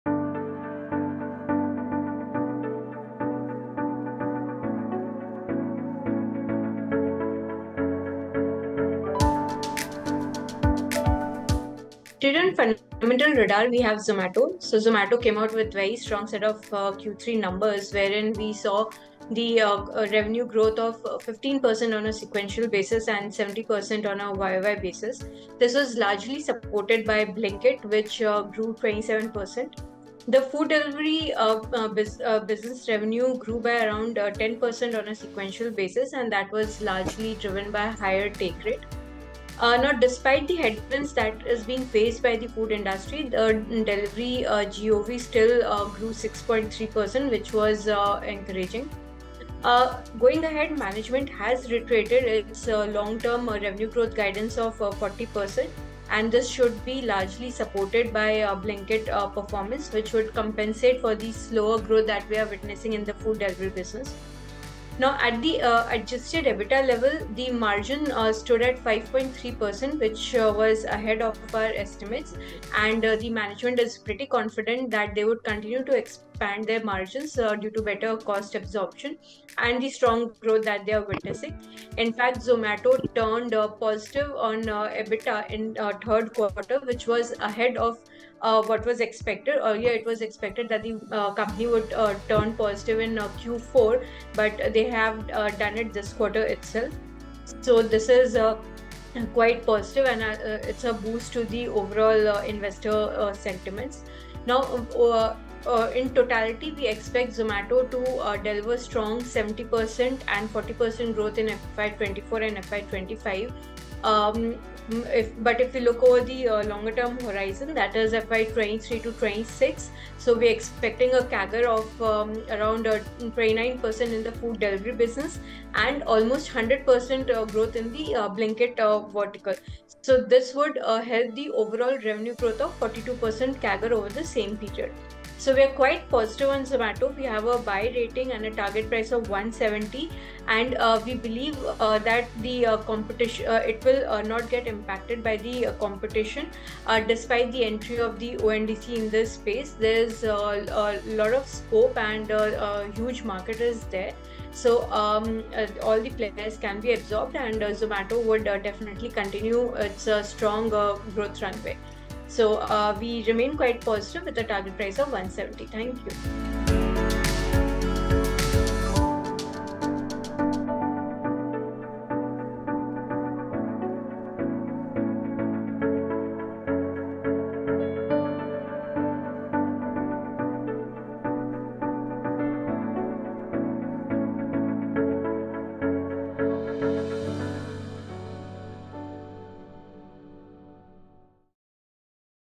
Fundamental Radar I Long-term growth prospects for Zomato intact despite ONDC entry: Sneha Poddar
Online food delivery aggregator Zomato reported a robust set of numbers for the quarter ended December, which beat analysts’ expectations. The company is set to deliver strong 70% YoY growth in FY24, with Blinkit outpacing the food delivery business because of geographical expansion, improvement in order frequency, and moderation in a competitive environment in the quick commerce industry, says Sneha Poddar of Motilal Oswal Financial Services.